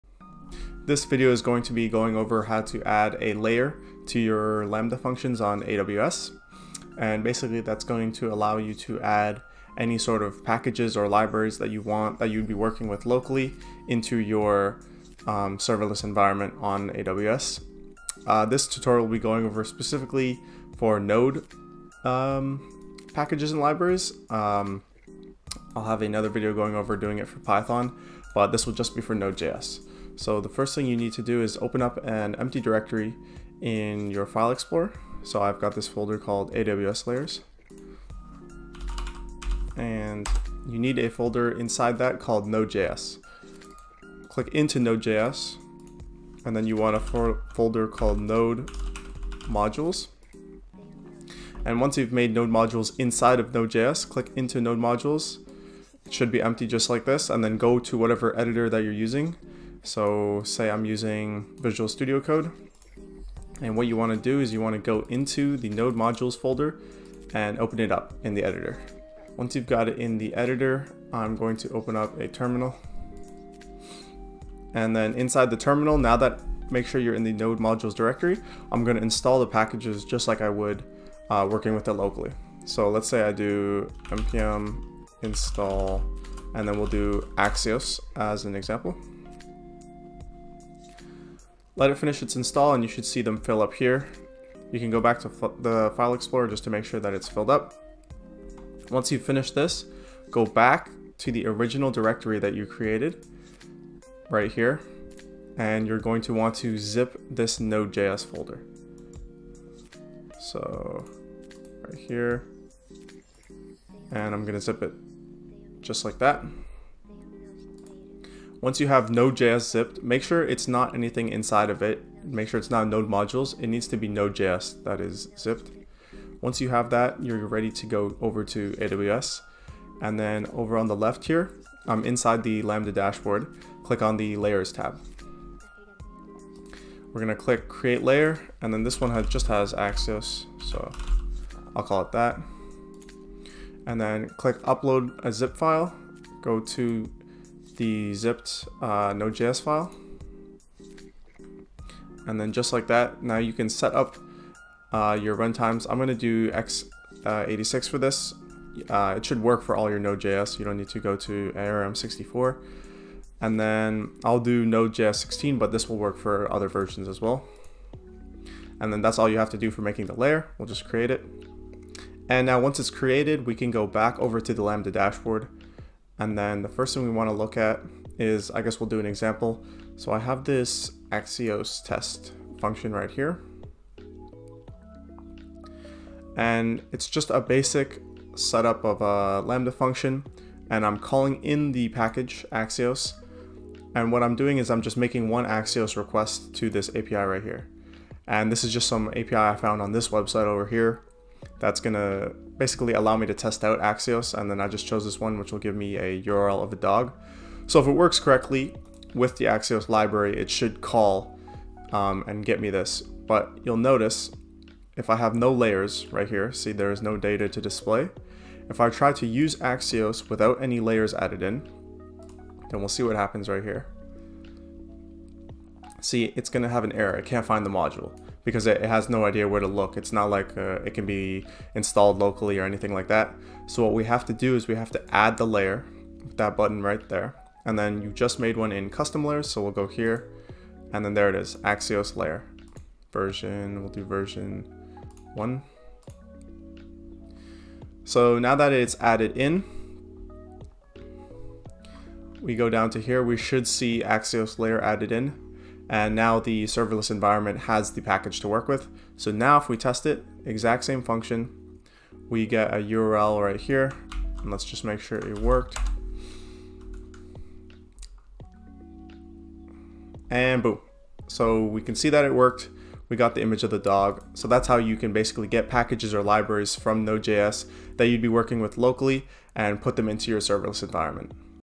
Using Layers to Add a Library to AWS Lambda!
To add libraries to lambda, there are a few extra steps that need to be taken in the form of lambda layers.  Lambda layers are a nice way of packing any library into an importable file that a serverless environment can access. Because there is no way to directly download or install libraries in a serverless environment, this is the best approach to take.

Lambda layers are quick to set up and in this video I go over the basics of installing a library and adding a lambda layer for node js.